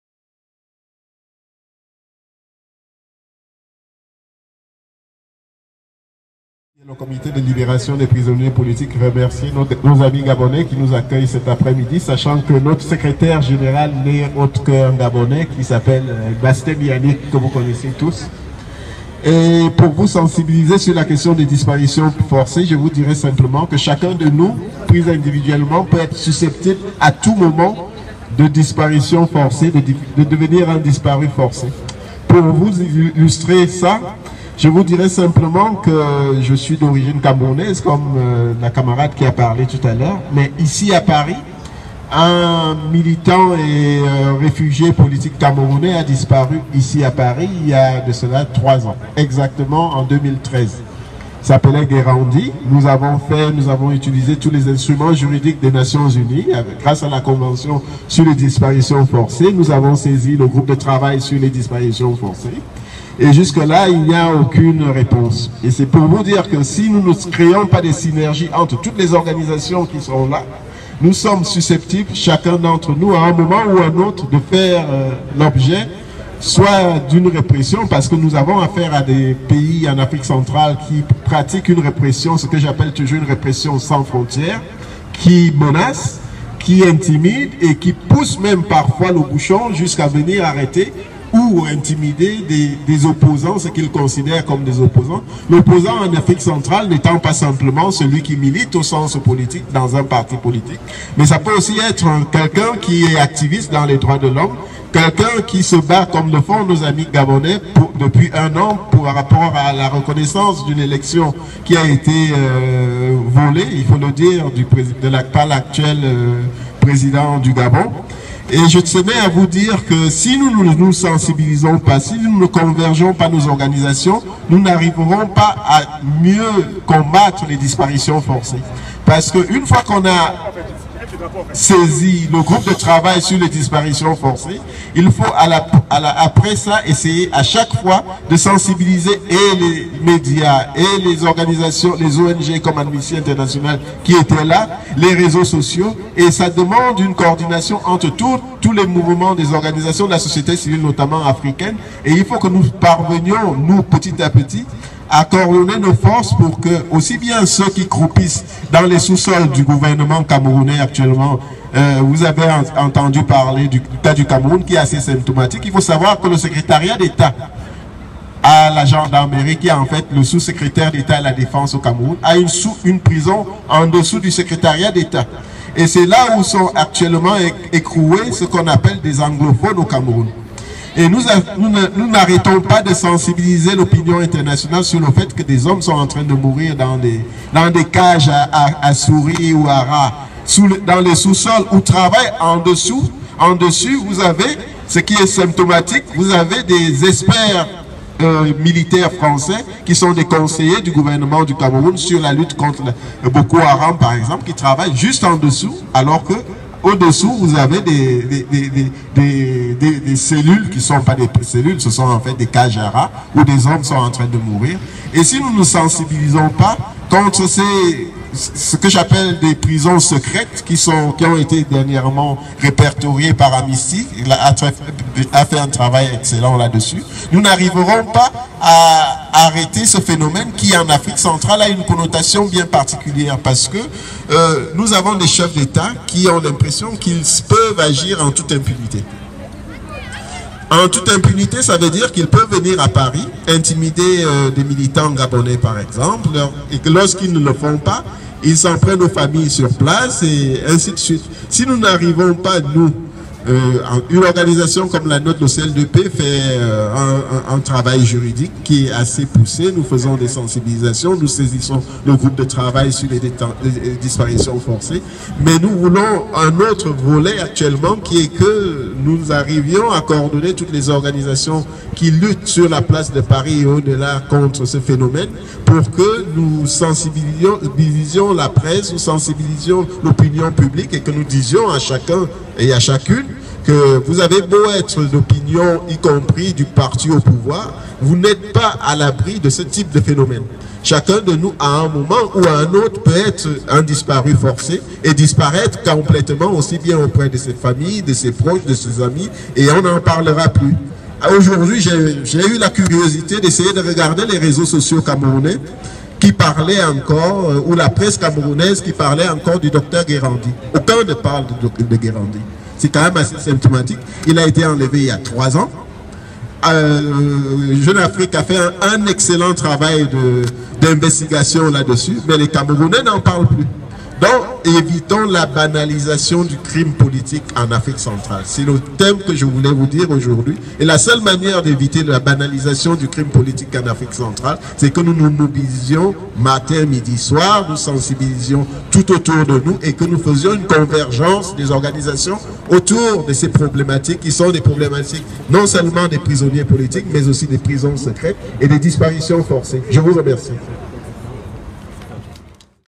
Journée internationale des victimes de disparition forcée - Joël Didier Engo (CL2P)
Intervention de Joël Didier Engo, président du Comité de Libération des Prisonniers Politiques (CL2P), lors de la manifestation organisée au Trocadéro (Parvis des Droits de l’Homme) à l’occasion de la Journée internationale des victimes de disparition forcée, le 30 août 2017.